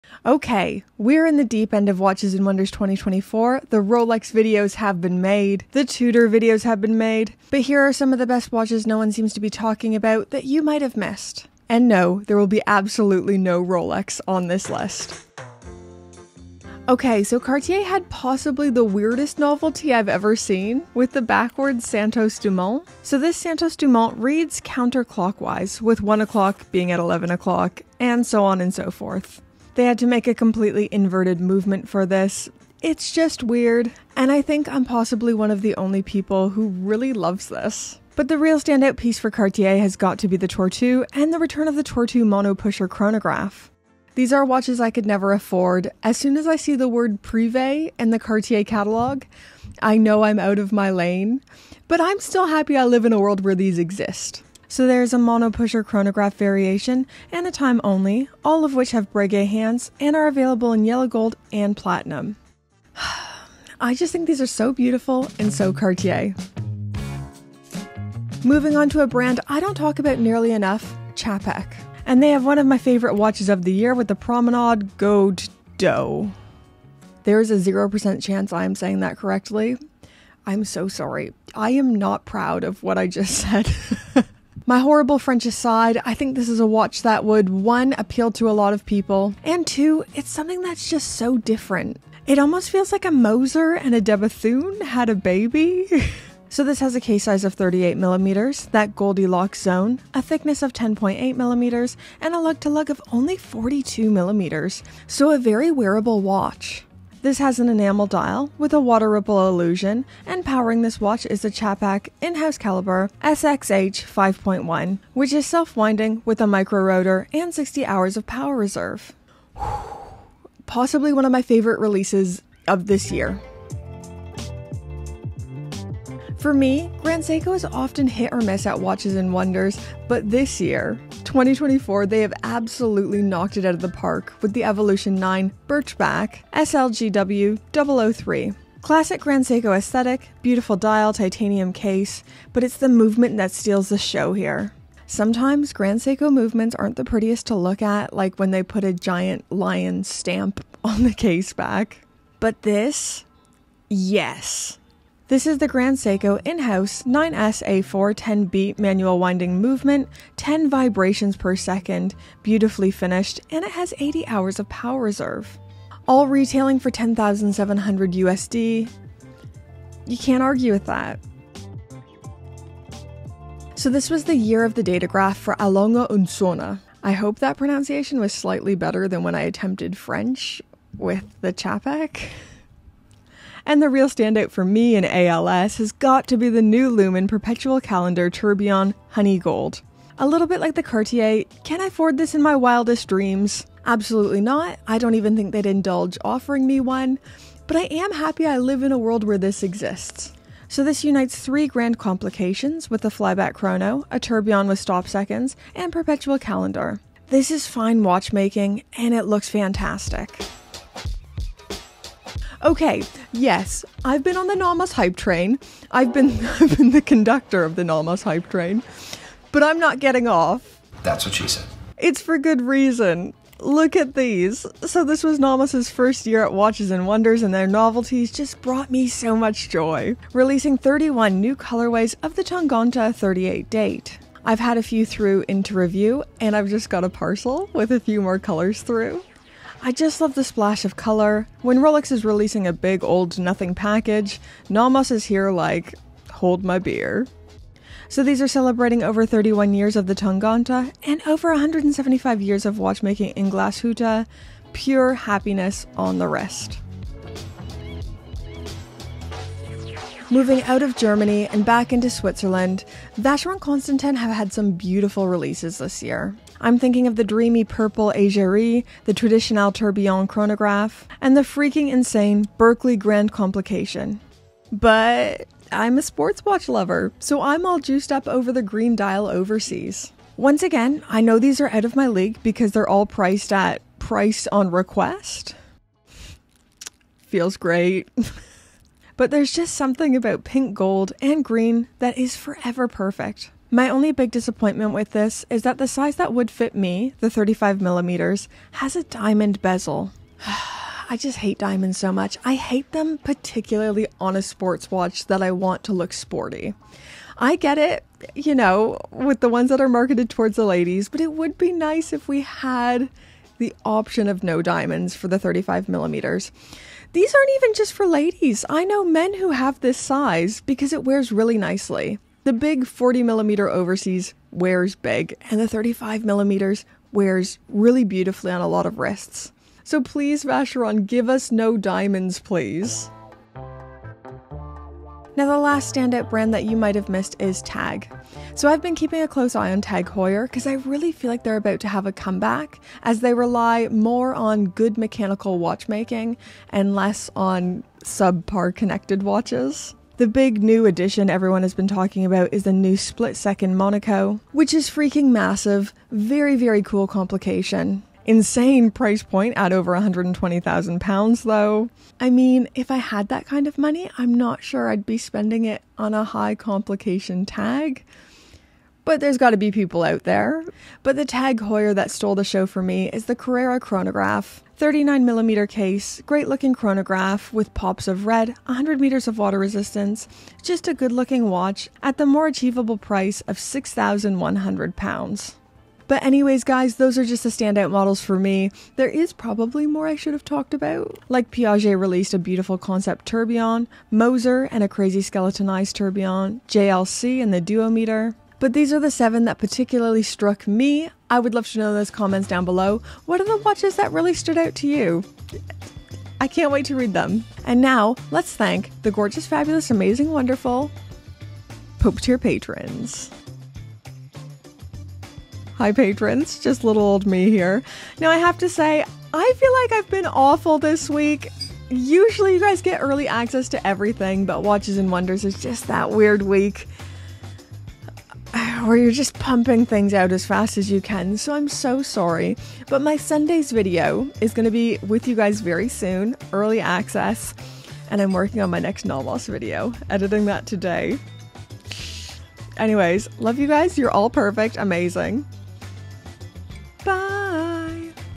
7 BEST NEW Watches You Missed This Year Because Everyone was Talking Rolex
We've got Cartier, TAG Heuer, Czapek, Nomos, Vacheron Constantin, AND SO MANY MORE who released not boring watches. Here are the 7 BEST WATCHES you might have missed this year because there are too many Rolex videos floating around (ok, whatever... I HAVE A ROLEX VIDEO ahah!)
My personal favourite has to be the new Czapek. But man, Cartier knocked it out of the park, so did ALS and VC! So many NEW WATCHES AND WONDERS WATCHES, AND SO LITTLE TIME!
7 BEST NEW Watches You Missed This Year Because Everyone was Talking Rolex

TIME STAMPS:
0:00-0:14 - Intro
0:15-1:05 - Cartier
1:06-2:02 - Czapek
2:03-2:50 - Grand Seiko
2:51-3:29 - A. Lange & Sohne
3:30-4:16 - Nomos
4:17-5:32 - Vacheron Constantin
5:33-6:30 - TAG Heuer
6:31-6:39 - Notable Shoutouts
6:40-6:53 - Outro
6:54-7:29 - Thank you Patrons!